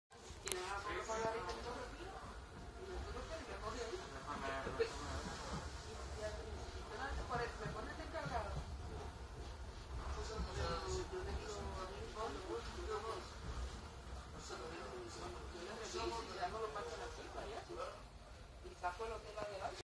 VELARO SERIE 103 EN DIRECCIÓN A ZARAGOZA Y BARCELONA SANTS GRABADO A LAS 17:33 MINUTOS DEL VIERNES
video realizado ayer viernes 29-1-21 a las 17:33 MINUTOS